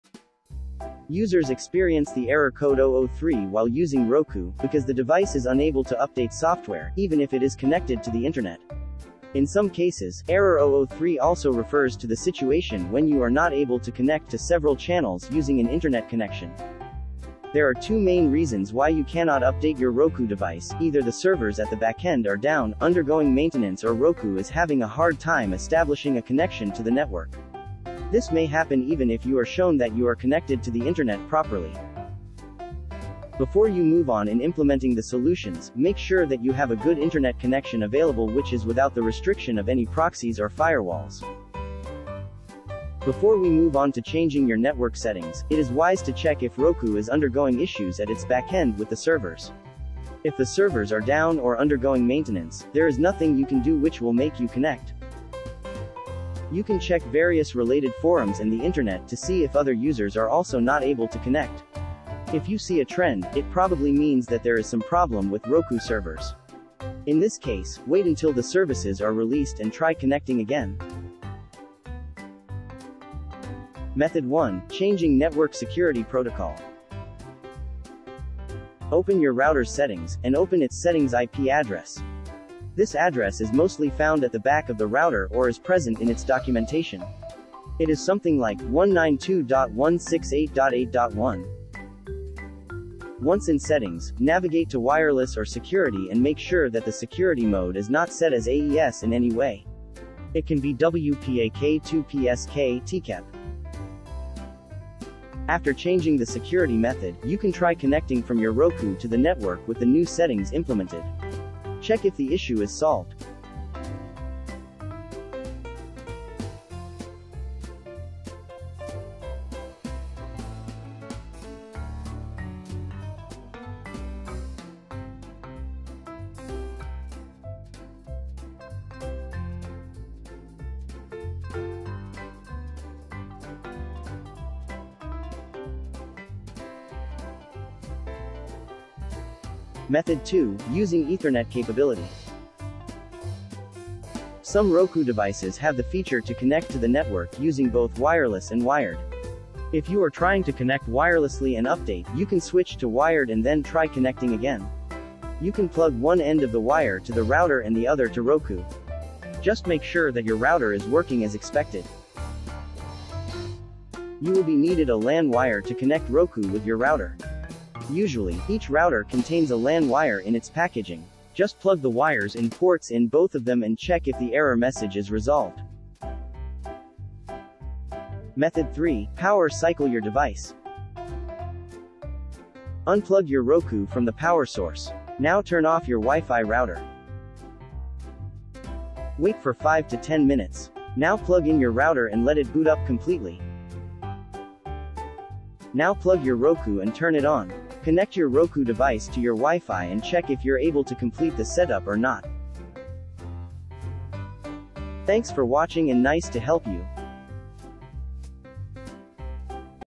3 Ways To Fix Roku Error Code 003 | Unable to update software
Roku Unable to update software. Please try updating again in a minute.